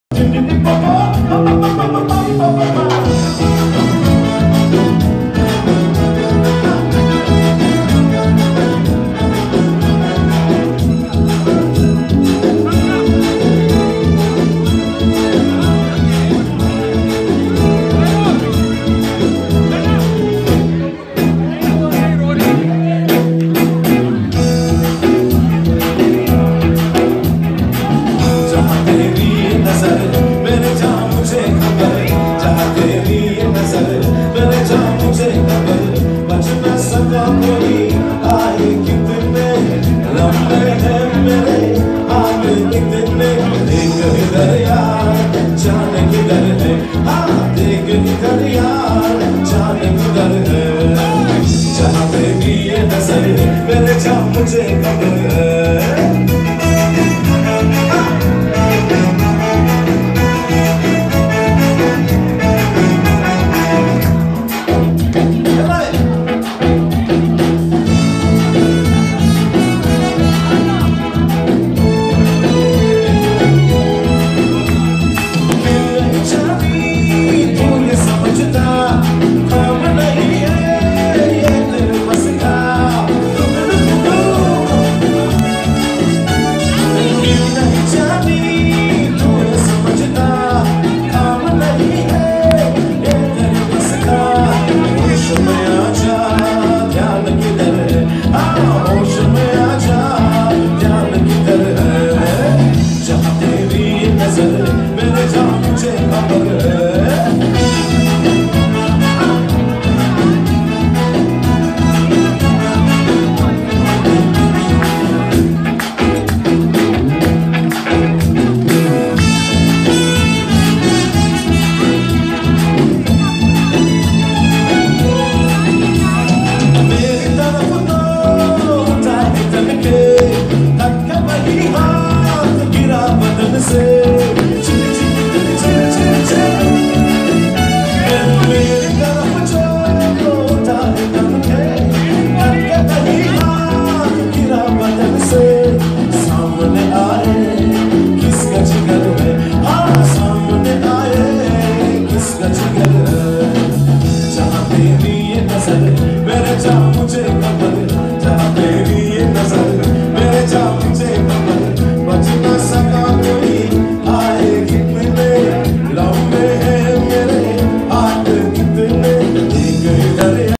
Sri Lankan wedding band "Backline" Hindi MContact (0712595718 Chaminda Suranjith)
Video from Chaminda Suranjith. Live recorded at Hilton Colombo